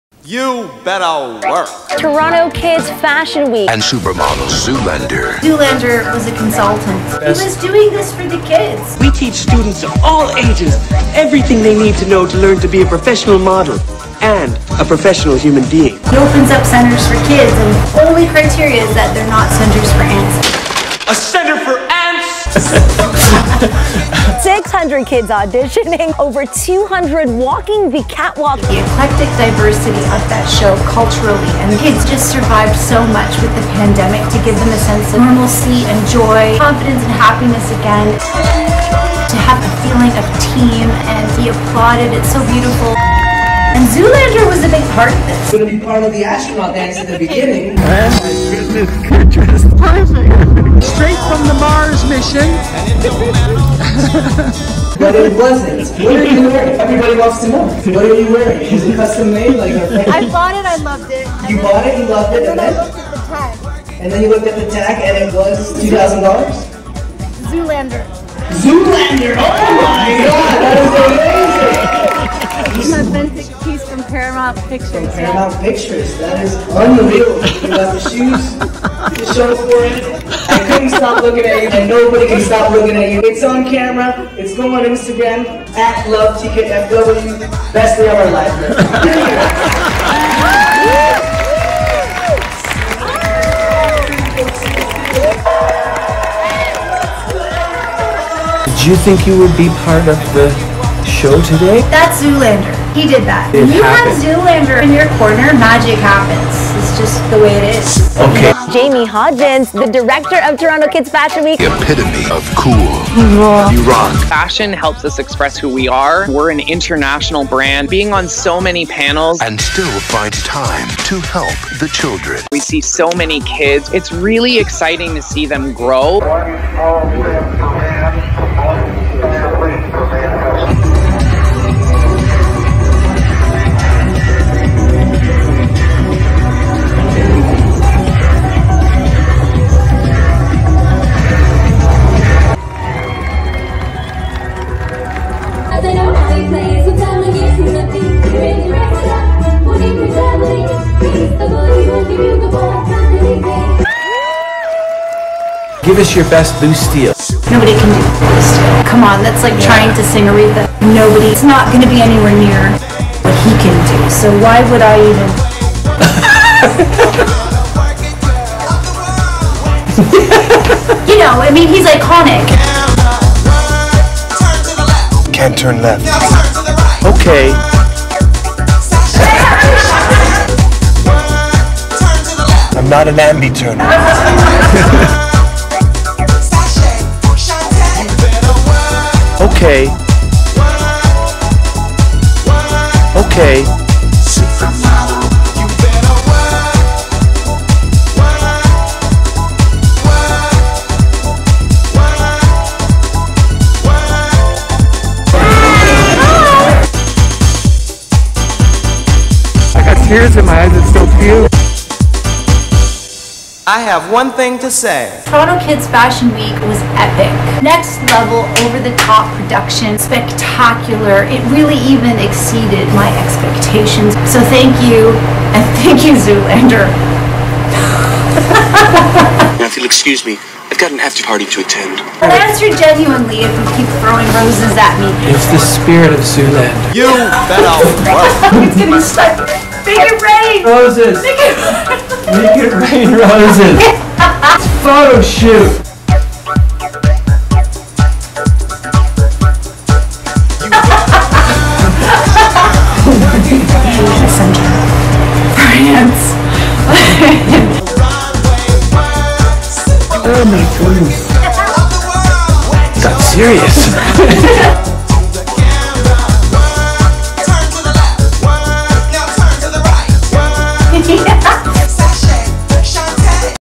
Carla Sacco - Toronto Kids Fashion Week December 2021
Zoolander parody of afflilate Toronto Kids Fashion Week (TKFW) Season 6 "Mission to Mars"!